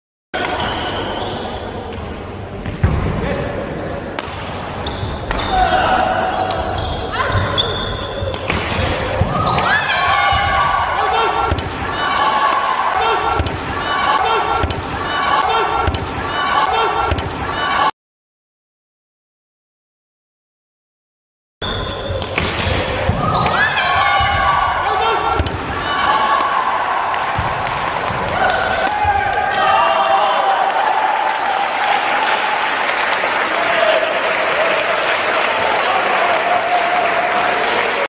2017 - Painful badminton faceplant during badmintonmatch
Diving after a badmintonshuttle is always at your own risk... Ouch!